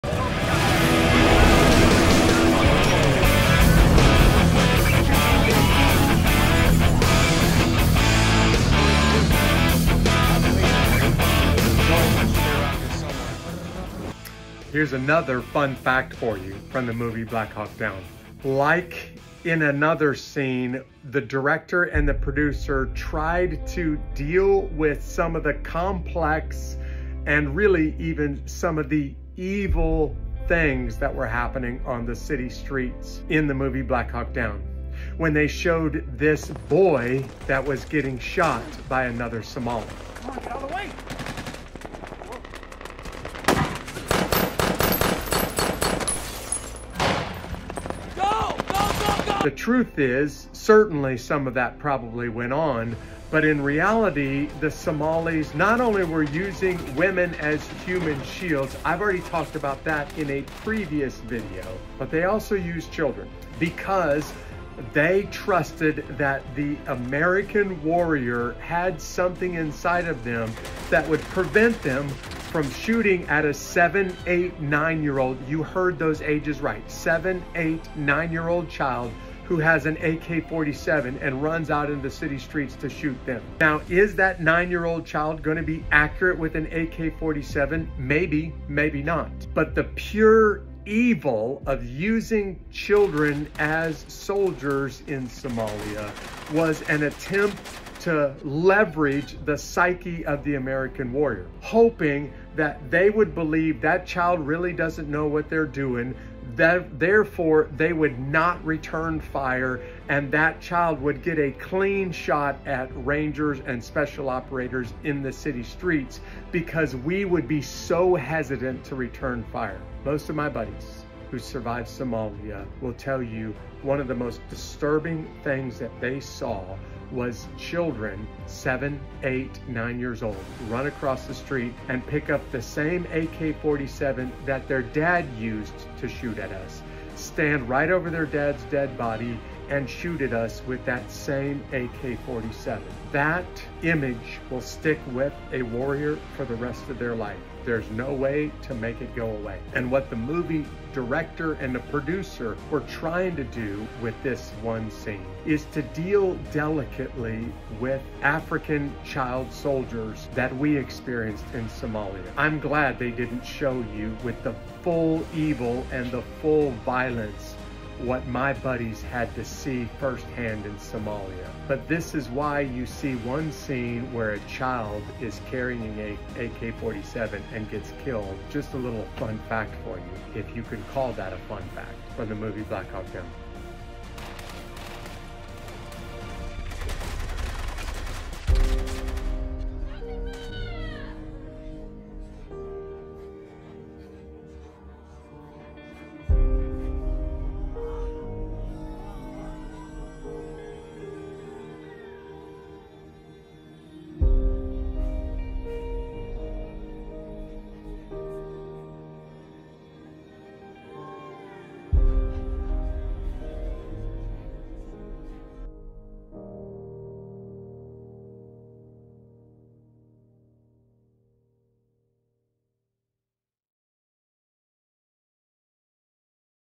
Black Hawk Down Fun Fact # 35 | Jeff Struecker
The movie hints at the use of child soldiers in Somalia. The truth was even darker, with kids as young as seven wielding weapons in the conflict. While the film addresses this issue delicately, it's hard to capture the full extent of the violence and evil. It's a haunting glimpse into the brutal realities of war.